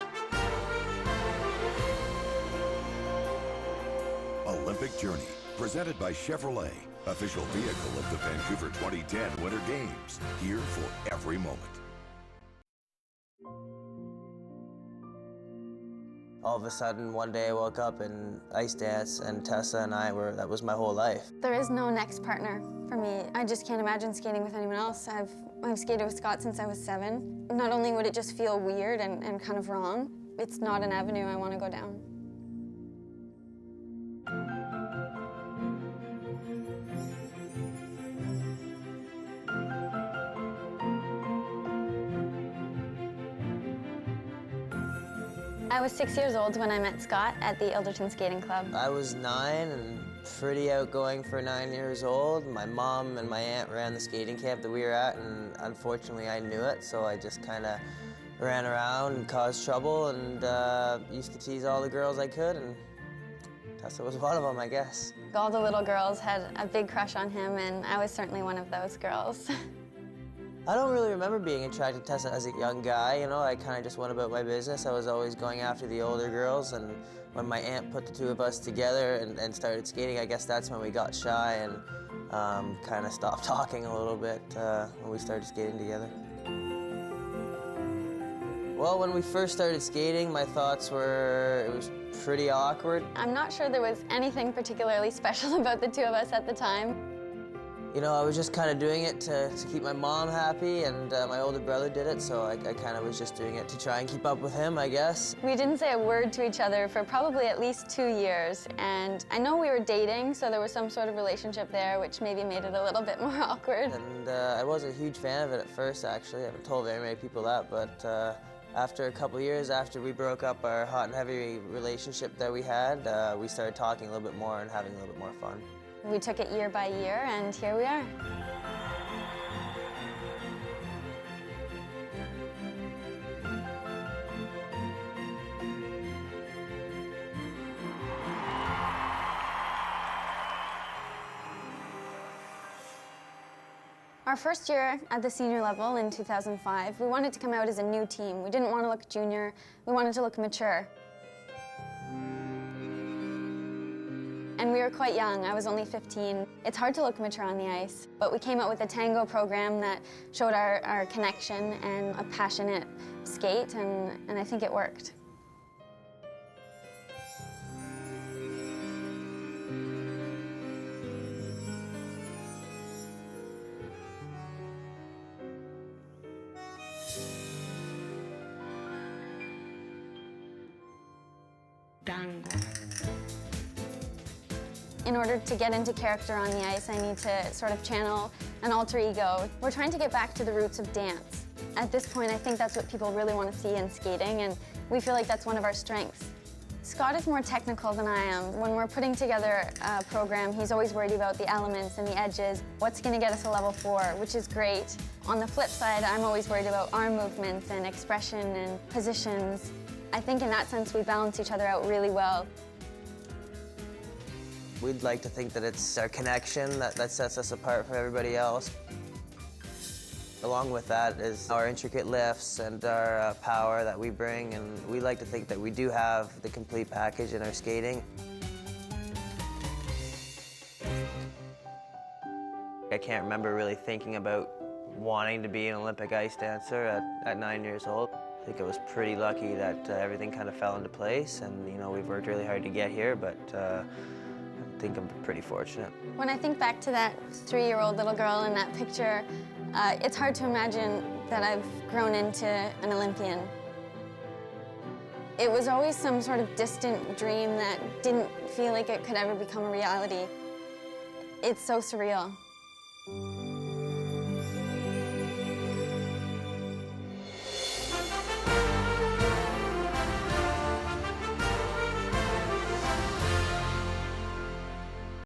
Virtue and Moir
The tv spot for Canadians Tessa Virtue and Scott Moir, Figure Skating Ice Dance Champions at the Vancouver 2010 Olympics.  The won the gold medal after their free dance on February 22, 2010.